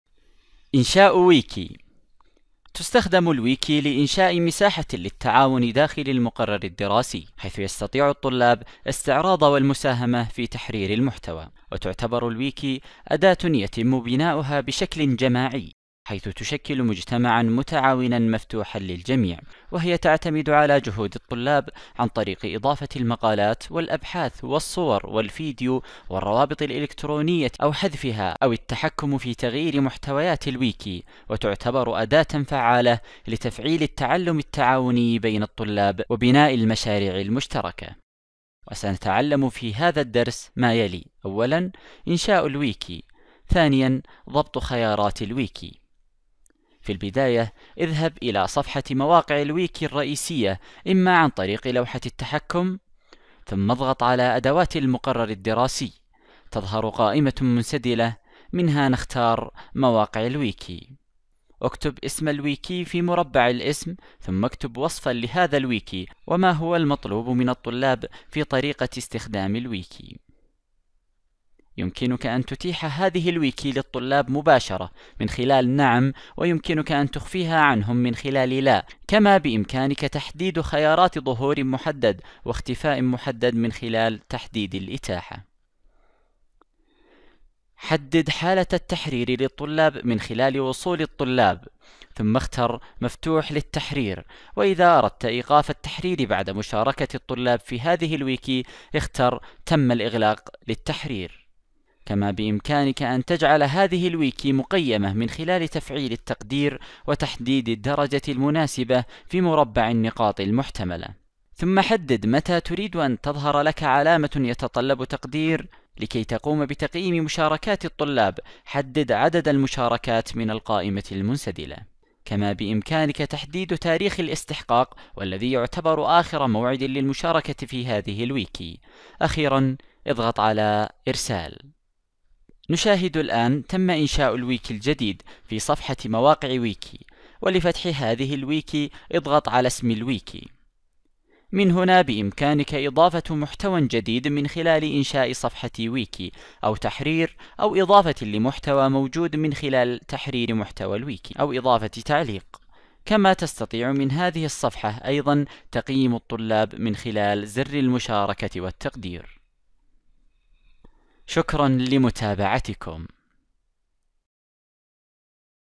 إنشاء ويكي
تستخدم الويكي لإنشاء مساحة لتعاون داخل المقرر الدراسي حيث يستطيع الطلاب استعراض والمساهمة في تحرير المحتوى
تعتبر   wiki أداة يتم بناءها بشكل جماعي ، حيث تشكل مجتمعاً متعاوناً مفتوحاً للجميع ، وهي تعتمد على جهود الطلاب عن طريق إضافة المقالات والأبحاث والصور والفيديو والرابط الإلكترونية أو حذفها أو تحرير بعض الكتابات أو التحكم في تغيير محتويات الويكي ,وتعتبر أداة فعالة لتفعيل التعلم التعاوني بين الطلاب وبناء المشاريع المشتركة.